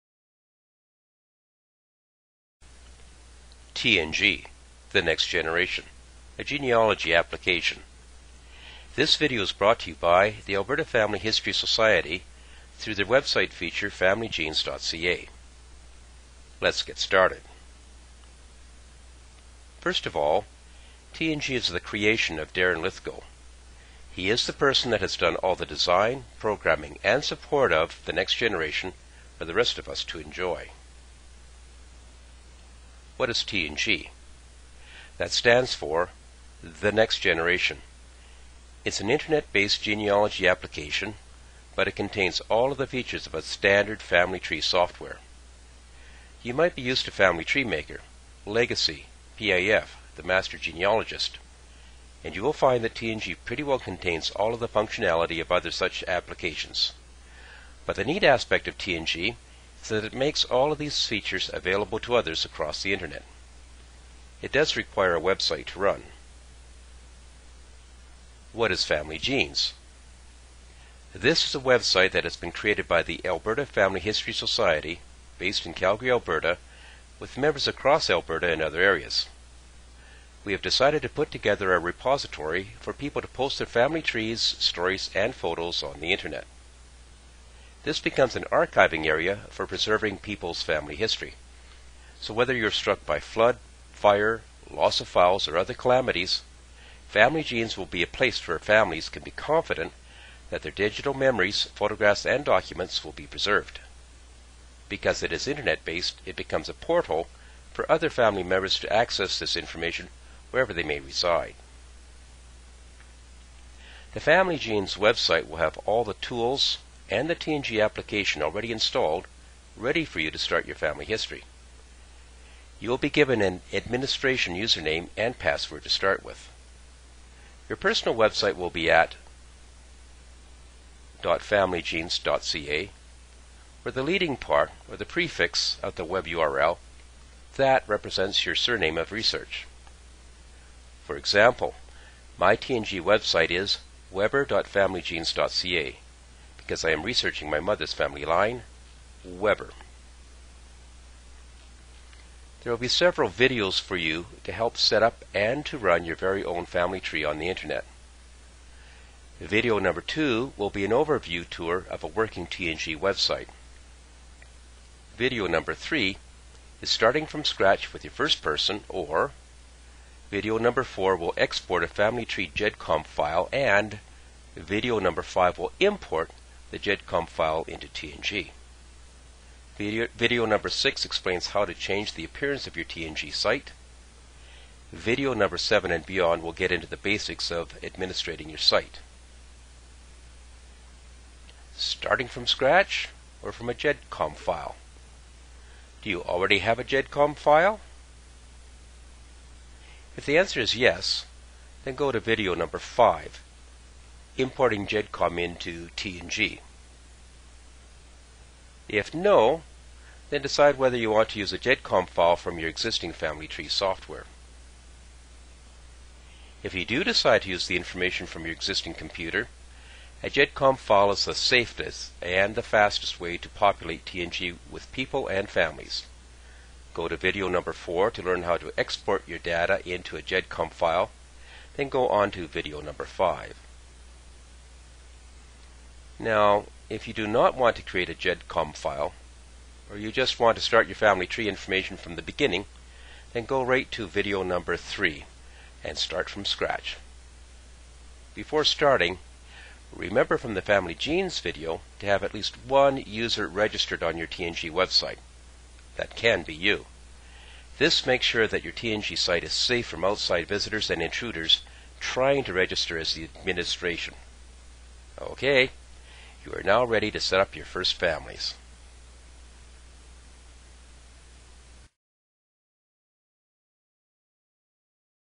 Video #1 - Getting started
An overview of "Th Next Generation", or TNG, a website application for managing your own family tree.  This is the first of several tutorial videos on this subject.